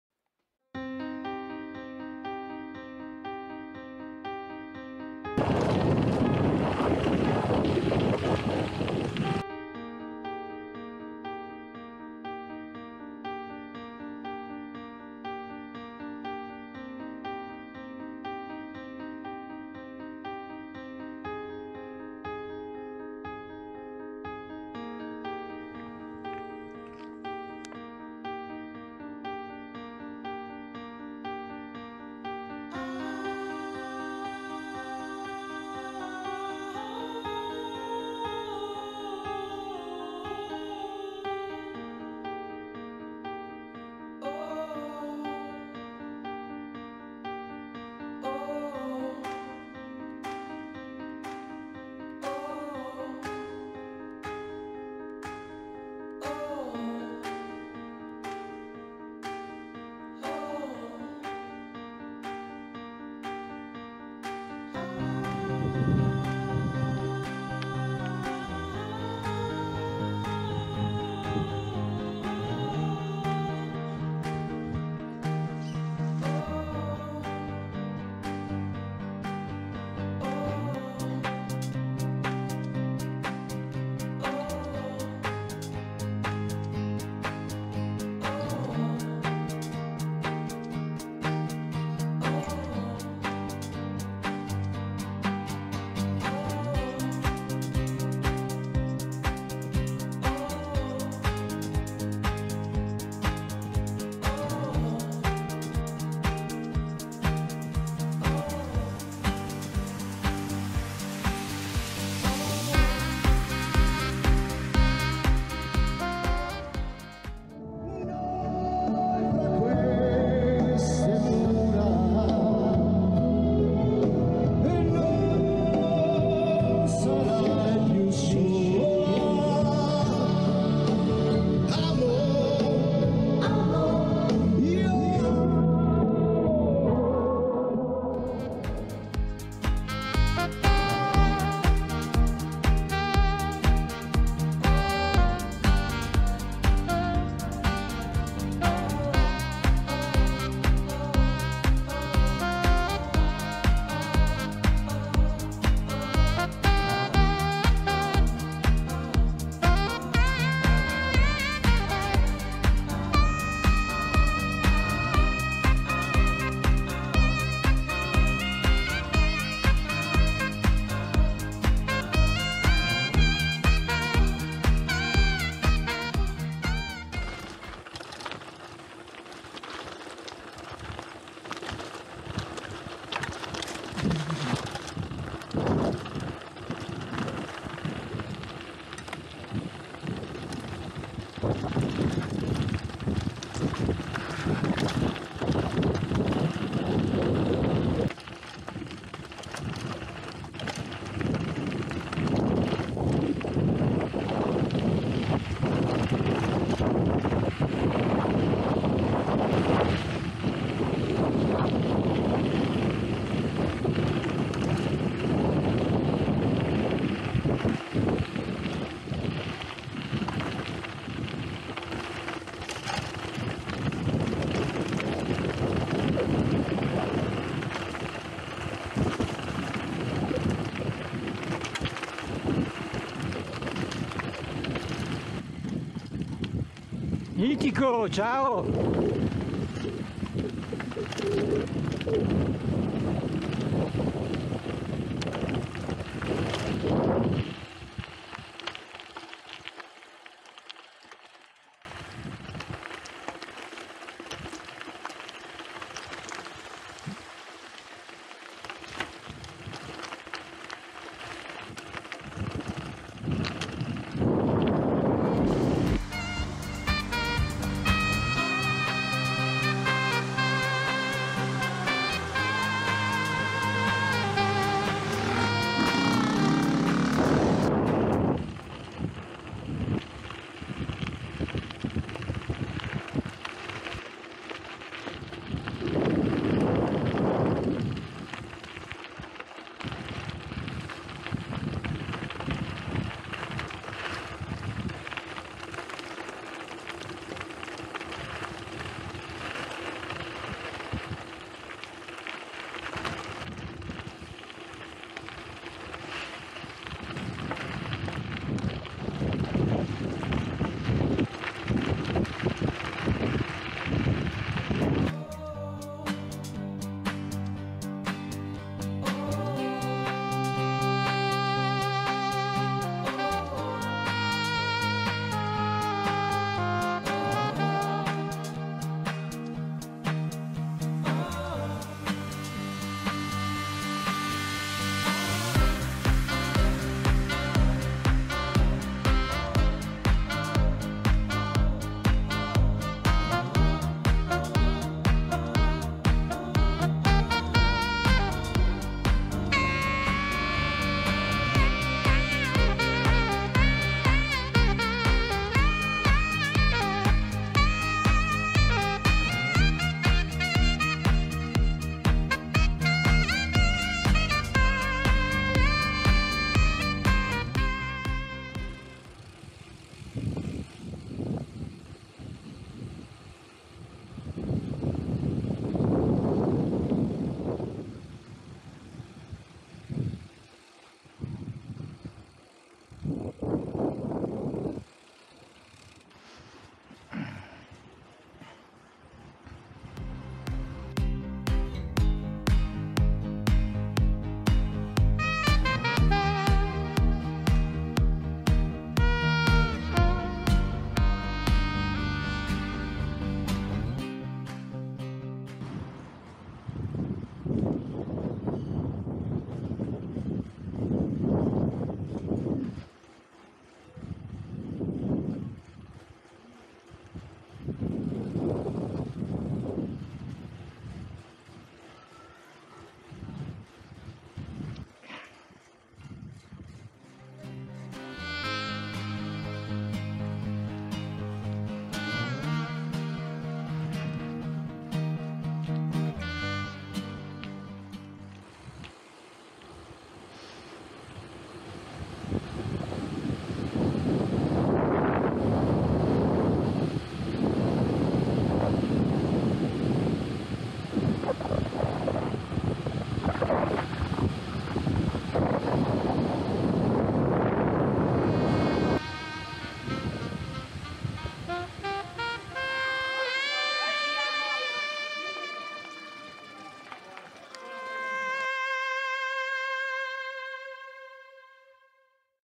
MTB - Dezzo - Dosso -Azzone - Paline - Borno - Parco Giovetto - Azzone - 15 agosto 2021
Partenza in MTB da Dezzo di Scalve. Prendo la strada che sale verso Borno passando per gli abitati del Dosso e di Paline. Prima di giungere a Croce di Salven entro al Lago Giallo a trovare Aldo e GianMario che stanno suonando e cantando aiutati da Fabio e Angela. Mi dirigo, poi, verso il Parco del Giovetto e dopo un lungo tratto non troppo impegnativo su strada a tratti sterrata e a tratti cementata, inizio la discesa verso Azzone. Faccio visita all'amico Robertino e torno al Dezzo dopo avere saluto Tino.